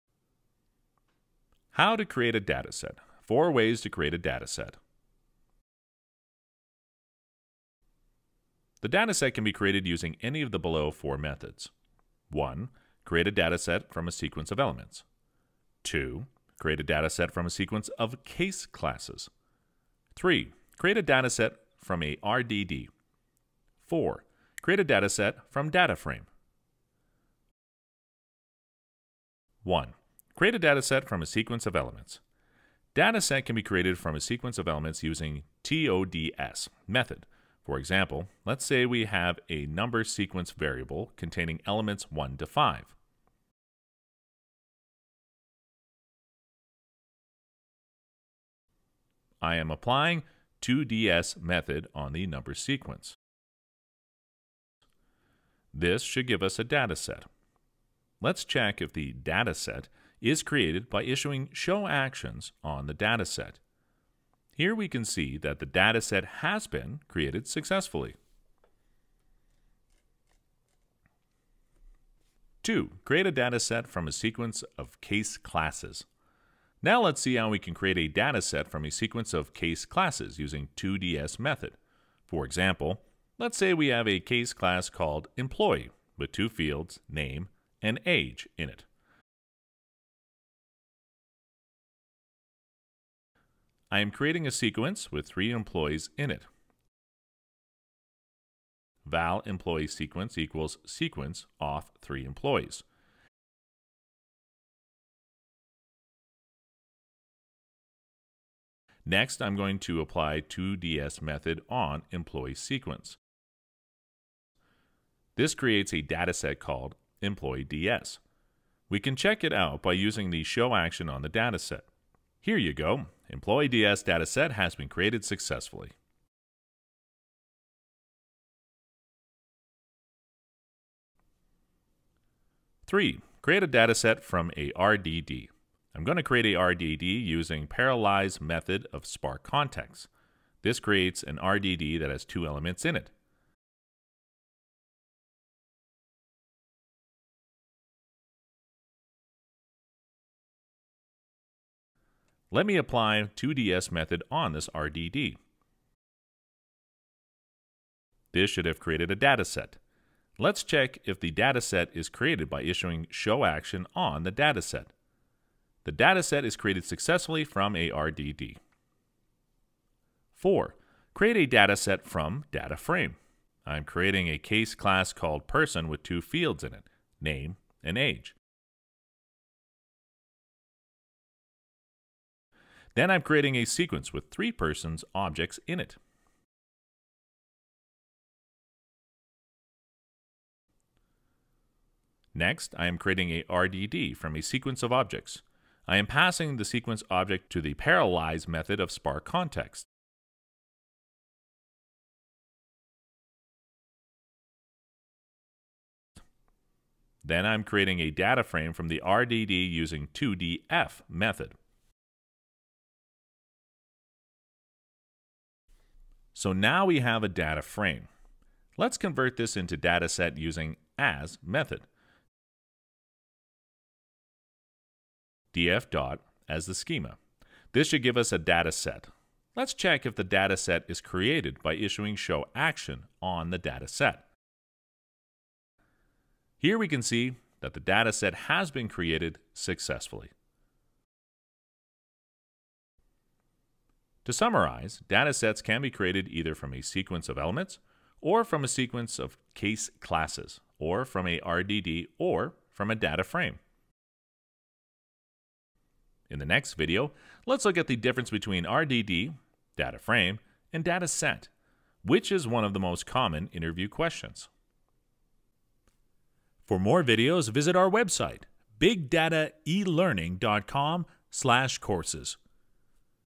How to create a Dataset in Spark : 4 ways to create a spark dataset
The dataset can be created using any of the below 4 methods.

1) Create a dataset from a sequence of elements
2) Create a dataset from a sequence of case classes
3) Create a dataset from a RDD
4) Create a dataset from Dataframe

1. Create a Dataset from a sequence of elements

Dataset can be created from a sequence of elements using toDS() method.  For example, let’s say we have a numberSeq variable containing elements 1 to 5.  Applying toDS method on the numberSeq should give us a dataset.

2. Create a Dataset from a sequence of case classes

Now let’s see how we can create a dataset from a sequence of case classes using toDS() method.  For example, let’s say we have a case class called “Employee” with 2 fields “name” and “age” in it. Creating a sequence with 3 employees in it. Apply toDS method on employeeSeq.  This creates a dataset called employeeDs.

3. Create a Dataset from a RDD

So far, we have seen how to create a dataset from sequence of elements or from a sequence of case classes.  Now, let’s see how to create a dataset from a RDD.  I am going to create a RDD using parallelize method of spark context.  This creates an rdd that has 2 elements in it.  Apply toDS method should create a dataset

4. Create a Dataset from Dataframe

Finally let’s try creating a dataset from a dataframe.  I am creating a case class called person with 2 fields in it, name and age.  Then I am creating a sequence with 3 person objects in it.

Creating a rdd, from a sequence of objects. I am passing the sequence object to the parallelize method of spark context. Then I am creating a dataframe from the rdd using toDF() method.  So now we have a dataframe.  Let’s convert this into dataset using “as” method.  Df dot as the schema.  This should give us a dataset.  Let me apply toDS method on this RDD.  This should have created a dataset.

To summarize, Datasets can be created either from a sequence of elements, or from a sequence of case classes, or from a RDD or from a Dataframe.